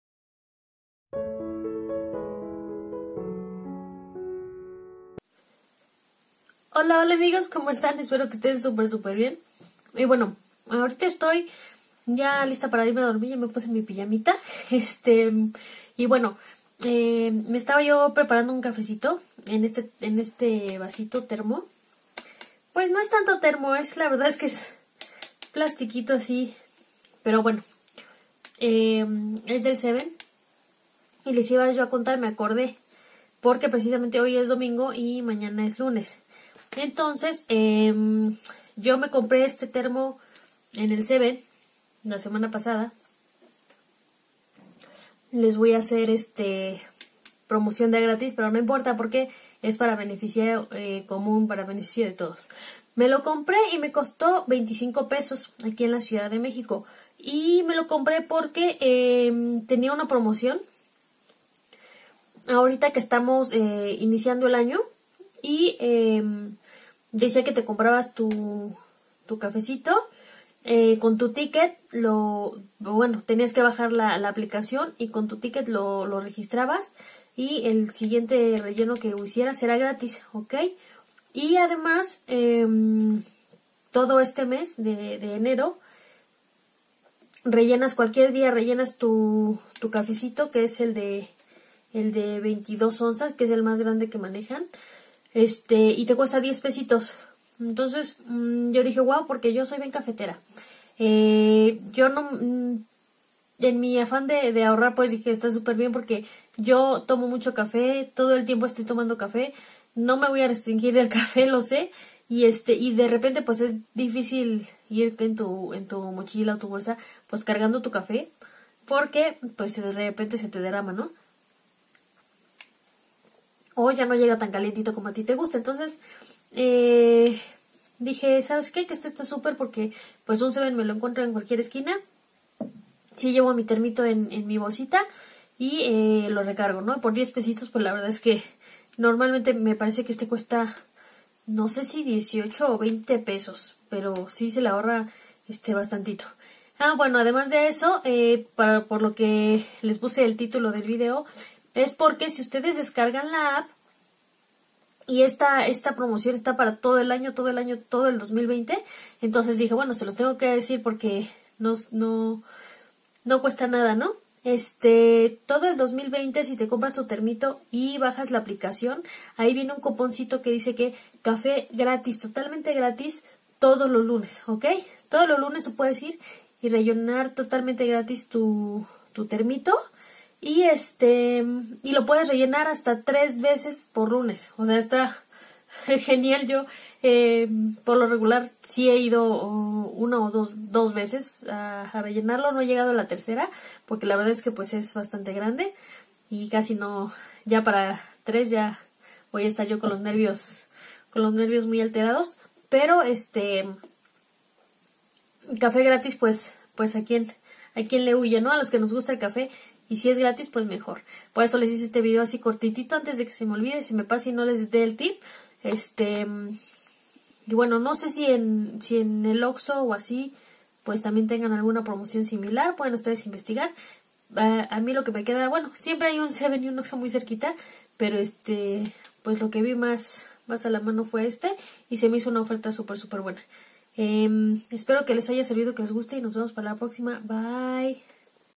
🔴 Café Gratis todos los lunes del 2020
Café Gratis todos los lunes en el Seven eleven Mexico, descarga la app, compra tu termo y disfruta cafecito gratis